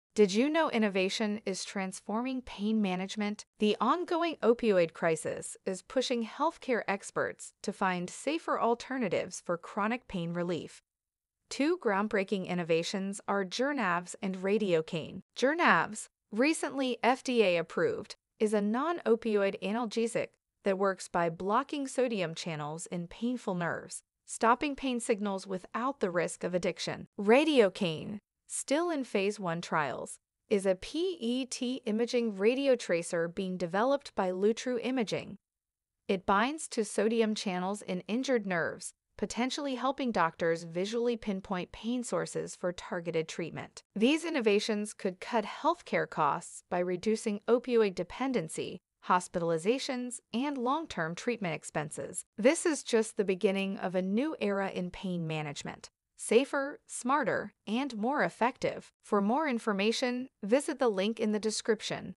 Innovation in Pain Management: Journavx and Radiocaine (one-minute video)
The ongoing opioid crisis has compelled numerous stakeholders in the healthcare industry to seek safer alternatives for managing chronic pain. Among the groundbreaking developments are Journavx, a recently FDA-approved non-opioid analgesic, and Radiocaine, an emerging PET imaging radiotracer currently in Phase 1 clinical trials. These innovations seek to offer new therapeutic and diagnostic options while potentially easing the economic burden imposed by chronic pain, opioid dependency, and related complications.